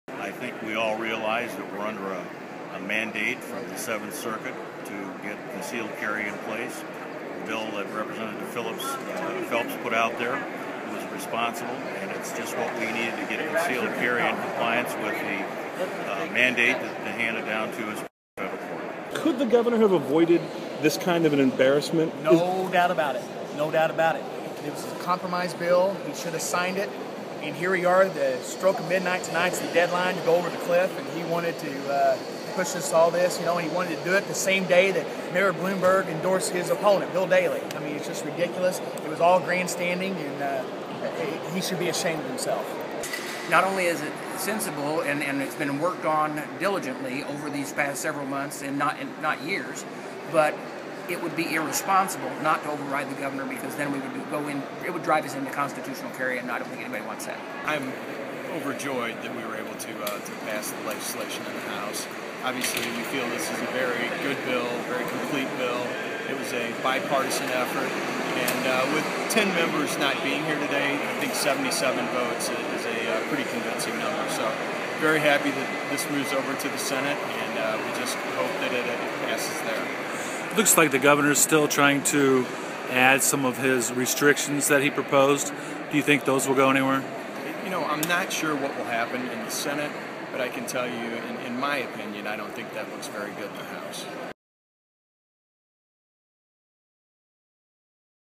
Conceal Carry Illinois
State representatives on the Illinois House passing a concealed carry bill. The House on Tuesday overwhelmingly rejected Illinois Gov. Pat Quinn's amendatory veto that put serious restrictions on where guns could be carried.